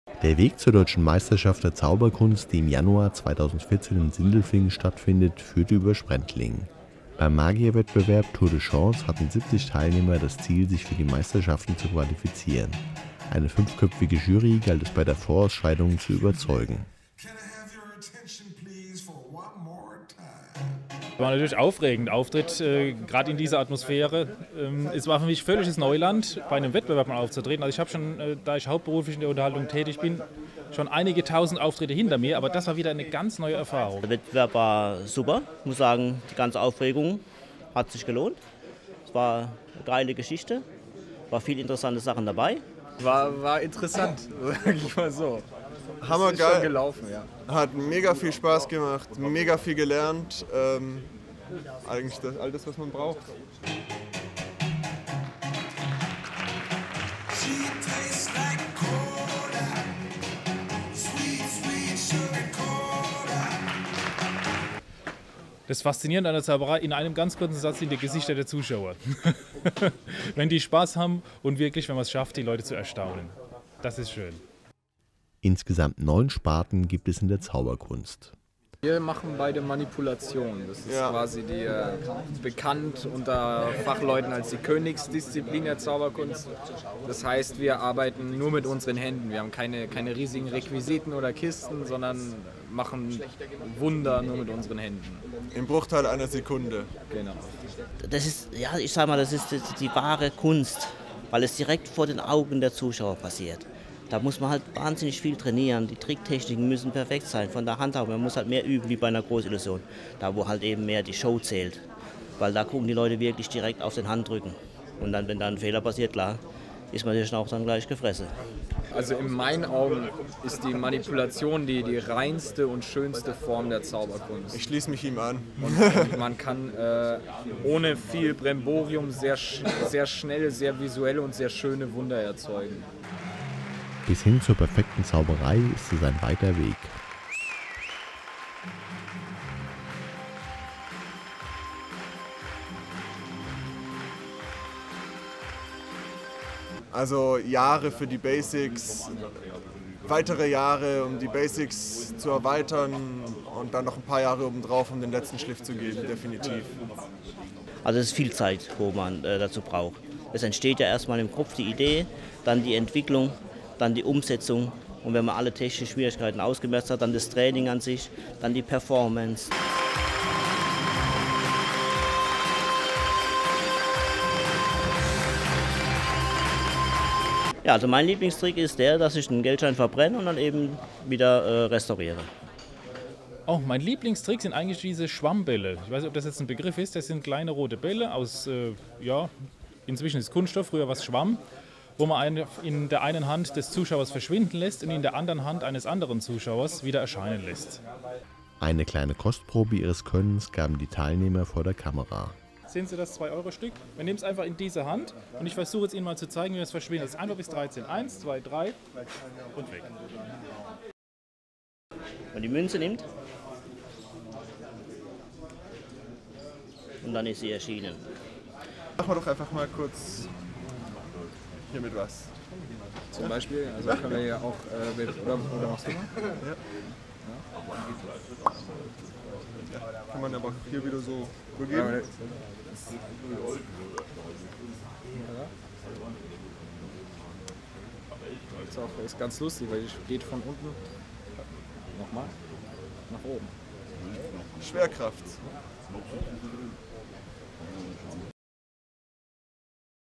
Zauberkunst in Sprendlingen
- 10. Juni 2013 - Beim Magier-Wettbewerb „Tour de Chance" im Bürgerhaus in Sprendlingen hatten 70 Teilnehmer das Ziel, sich für die Deutsche Meisterschaft der Zauberkunst zu qualifizieren.